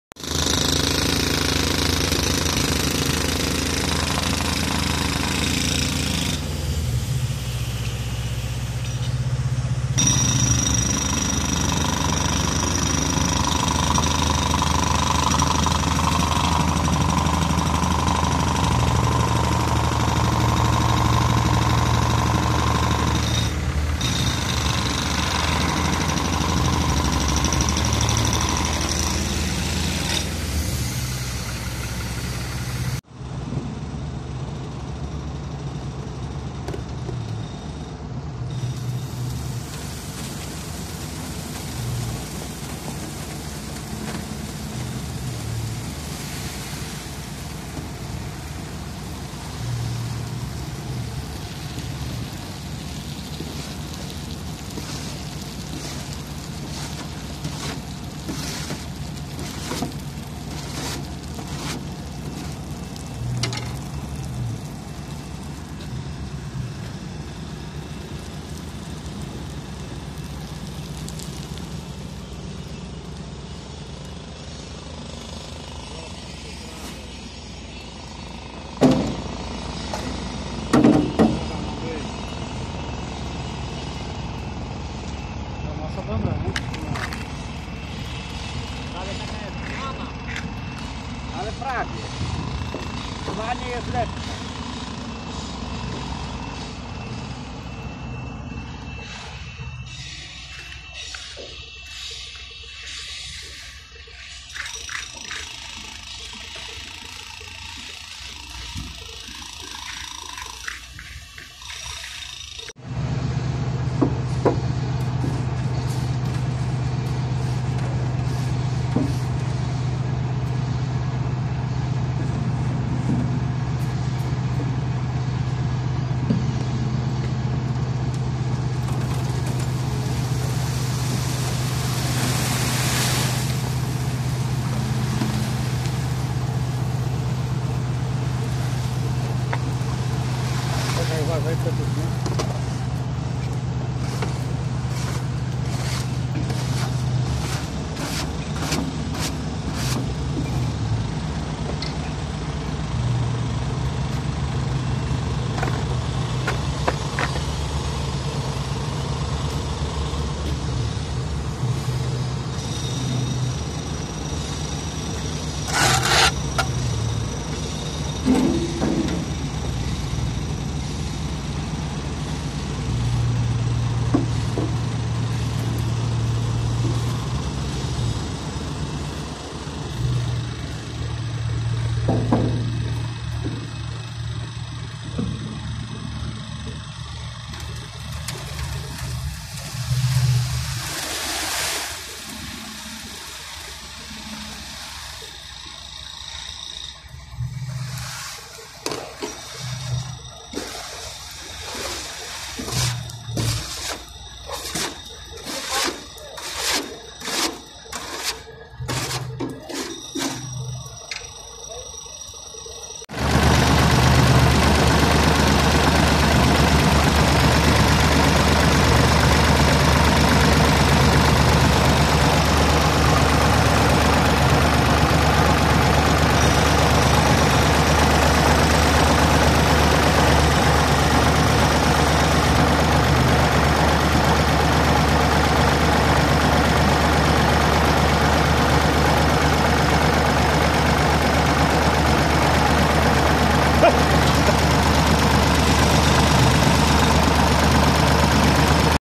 PETRO-NAFT JASŁO/TRANS-WIERT JASŁO/ZURP KOGA GDAŃSK, NAPRAWA DROGI ASFALTOWEJ
Dnia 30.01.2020 roku firma PRTG Petro-Naft s.c. Jasło, TRANS-WIERT Jasło oraz ZURP KOGA Gdańsk
Wykonała naprawę drogi asfaltowej, łatami z asfaltu twardolanego.
Prace wykonywane były na ulicy Łozy w Gdańsku.
Będzie tego o wiele więcej
Pozdro;-)